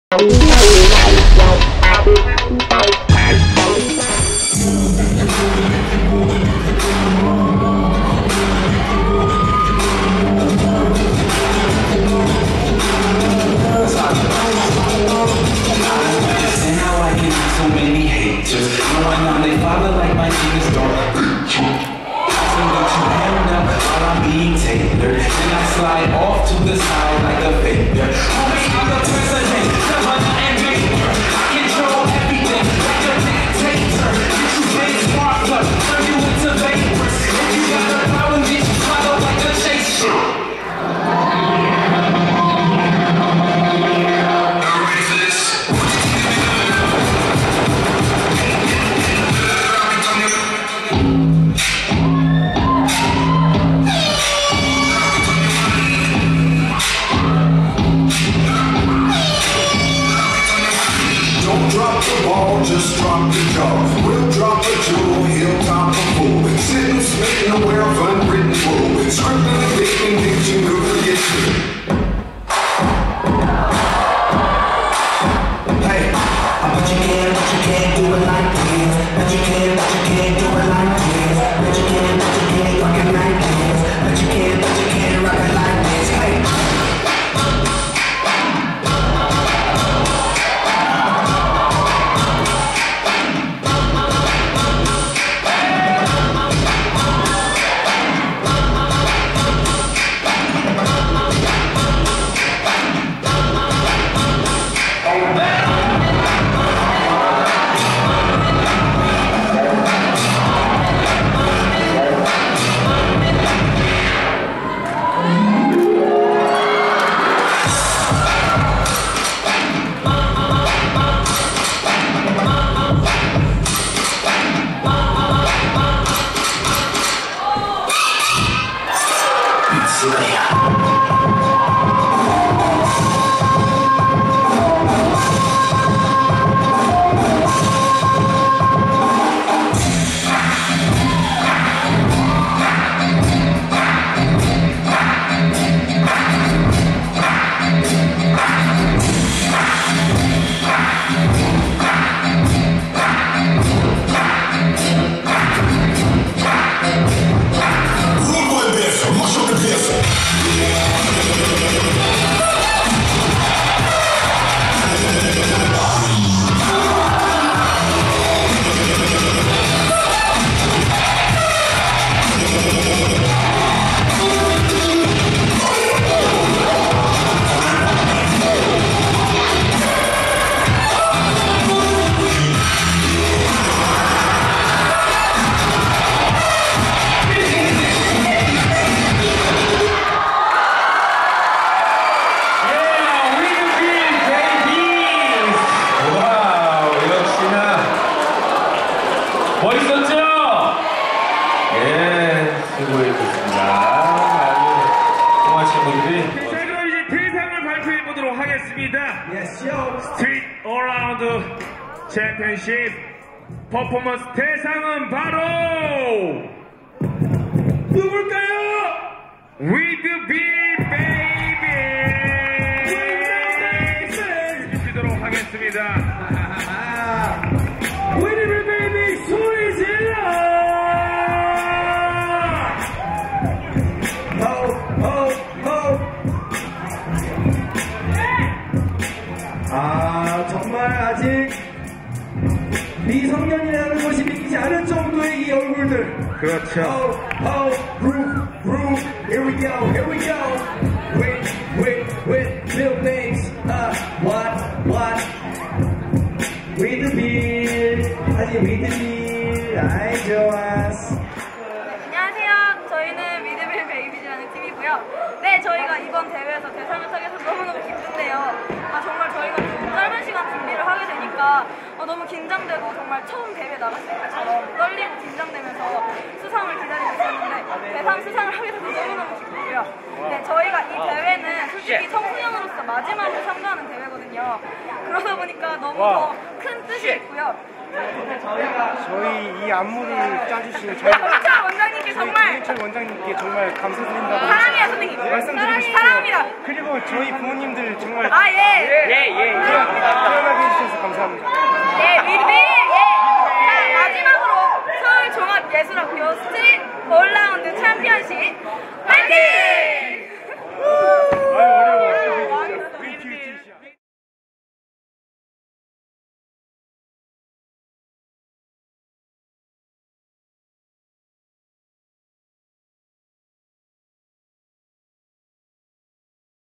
[싹튜브] 2014 Street All-round Championship Performance
서울종합예술학교 공식SAC 올 라운드 챔피온쉽대회
2014 Street All-round Championship Performance

서울종합예술학교와 The K 서울호텔이 함께했던 '2014 Street All-round Championship Performance'

이들은 상금 50만원과 함께 서울종합예술학교 4년장학(3200만원 상당) 혜택을 받지!

중,고등부문 대상 '위드빌베이비즈'의화려한 포퍼먼스를 만나보시라~!

더 놀라운게 뭔지 알어?
이거 1인당 3200만원이다!